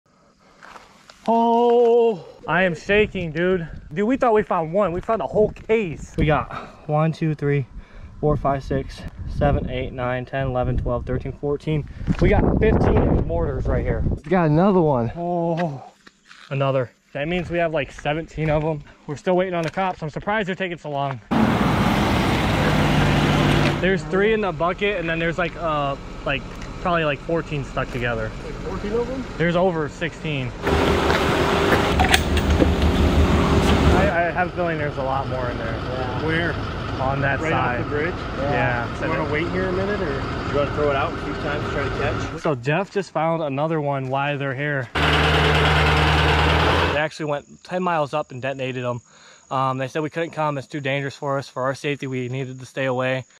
Bomb Squad Couldn’t Believe Their Eyes
Bomb Squad Couldn’t Believe Their Eyes When We Found This Magnet Fishing!